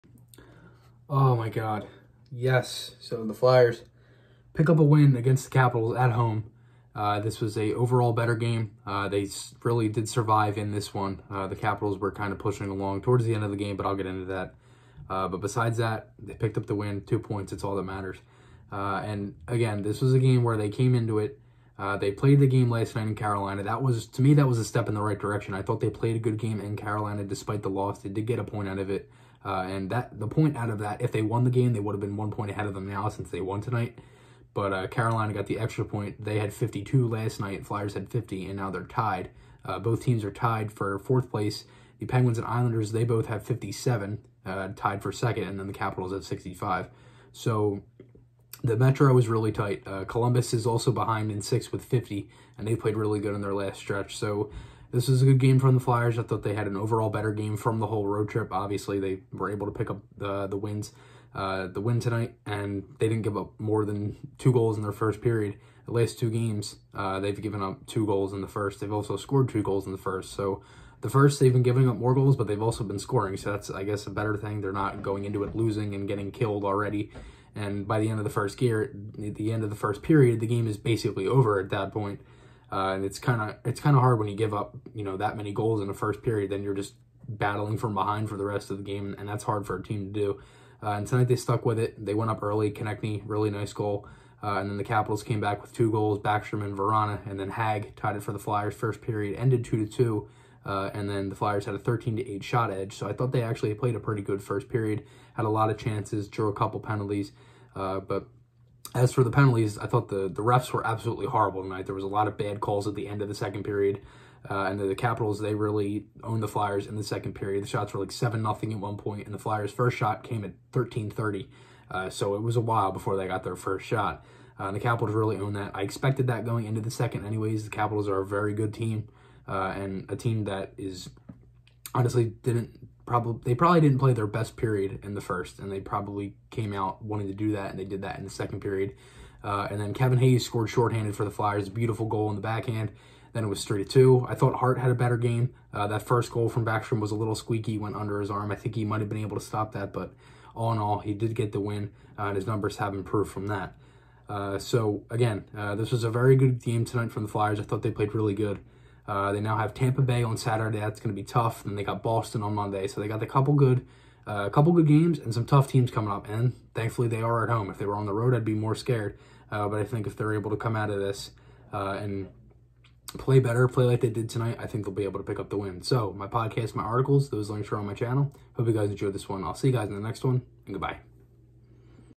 Home Sweet Home? Flyers 3, Caps 2
Better overall game from the Flyers tonight, really liked what I saw from the PK. Got some tough games coming up, grind em out.